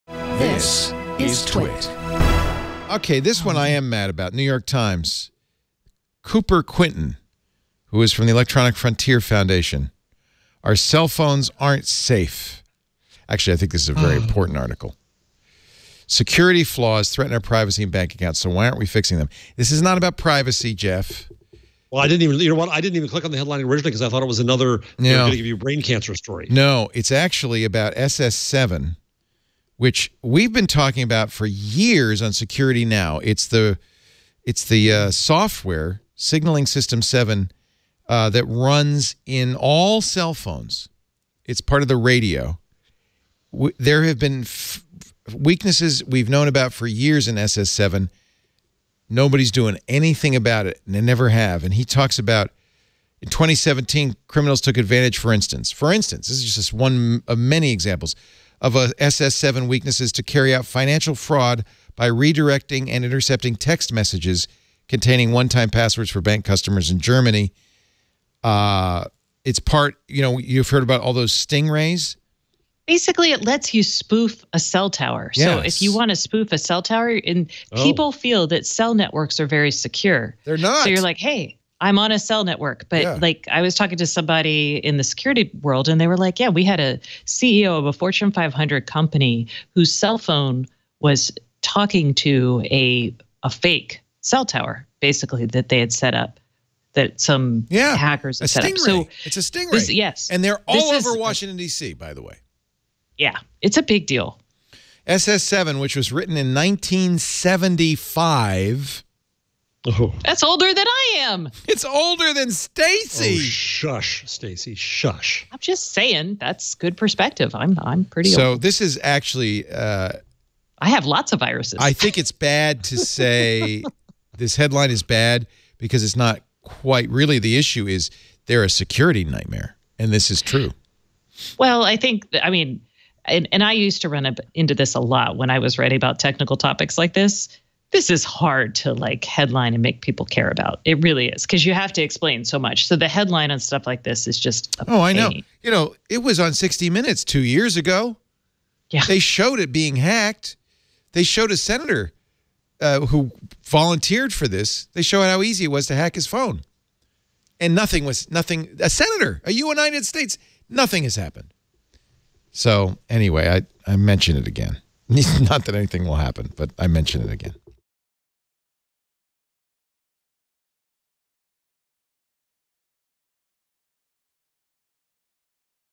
The Biggest Security Flaw in Your Cell Phone
The biggest security flaw in your cell phone has been there for decades, and there is no plan to fix it.